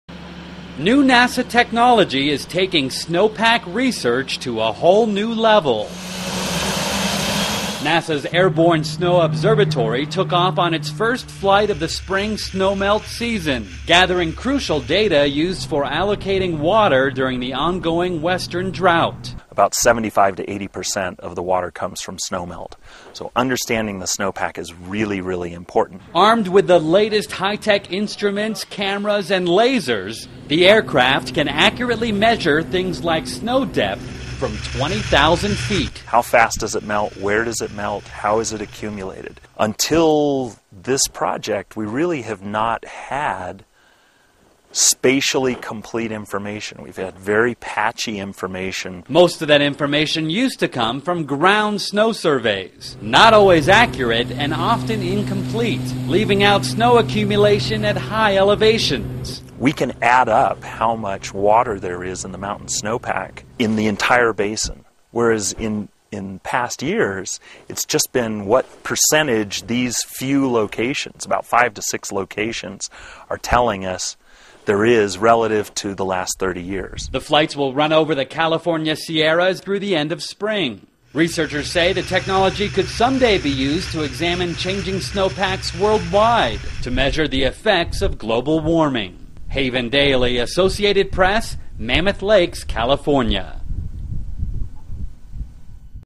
NASA Launches Mission to Measure Snowpack
A new NASA airborne mission is measuring the snowpack of major mountain watersheds in California. It's producing the most accurate measurements to date showing just how bad the drought may become. (March 26)